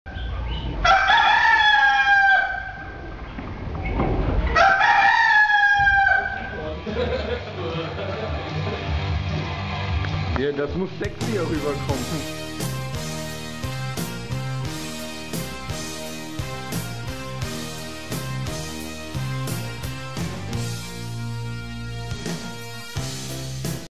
Raketenklee FanClub präsentiert: Ray Clover - Sexy Strip
Ein kleiner Strip von Ray Clover kurz vor dem MOM-Radio live Konzert am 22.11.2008 in Weilburg. Raketenklee sind: Bidi, Marc und Ray Clover.